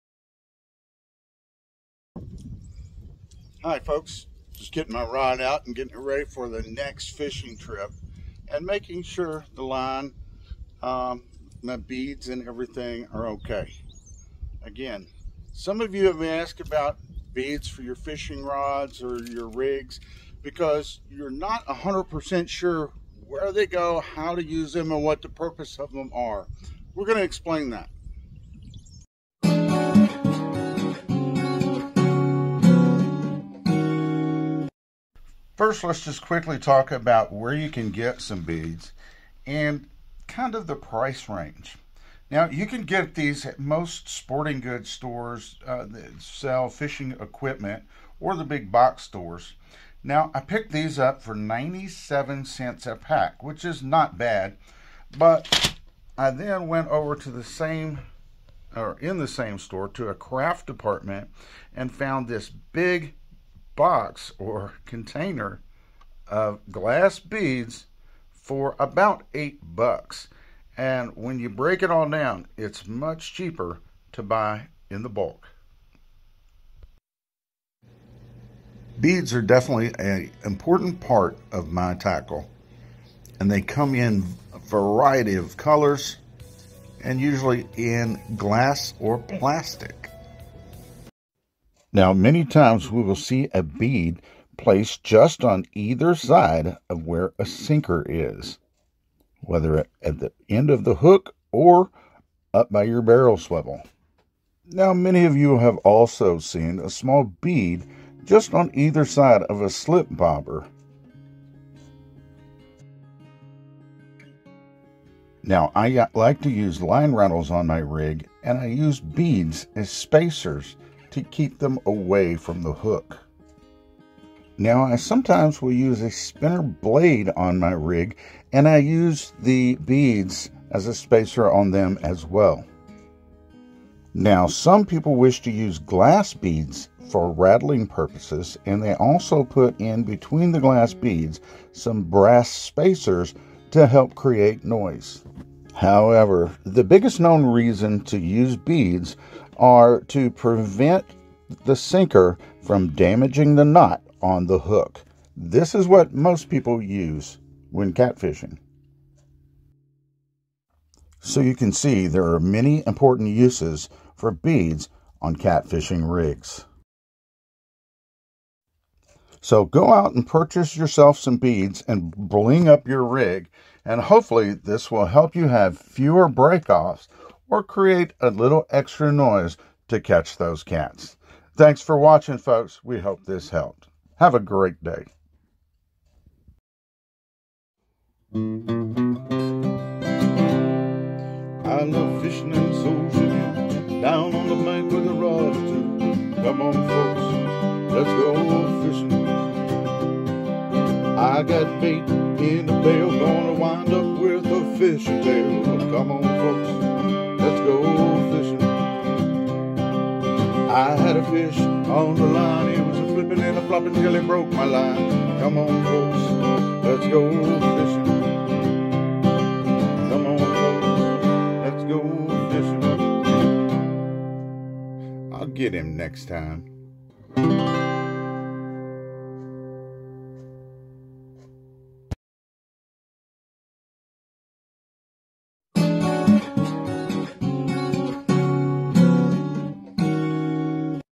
How to use beads for catfishing
Brad shows many different ways to use beads for catfishing rigs